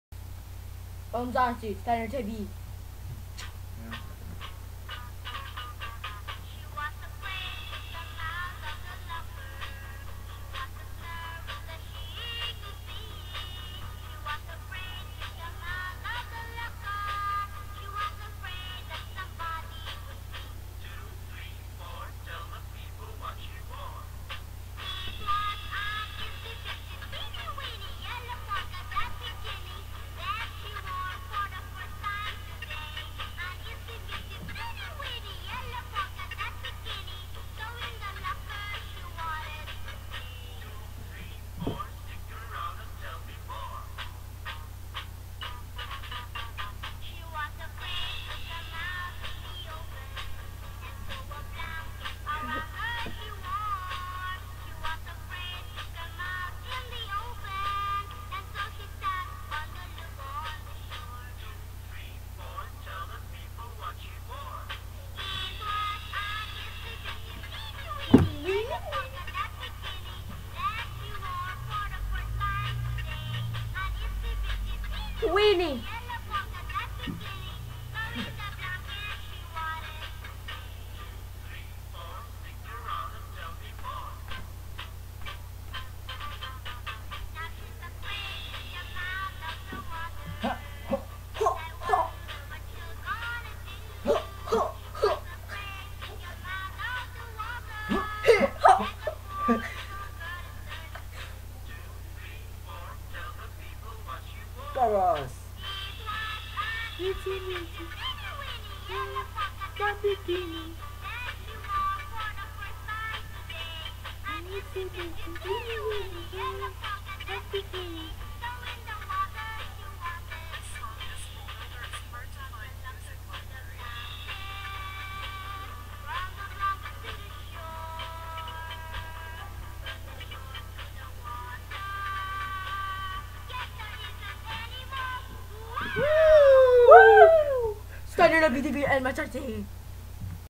SteinerNaviTV with elmzarnsi Lipsync
Me and My Brother (elmzarnsi) were bored so we decided to do a Little tune!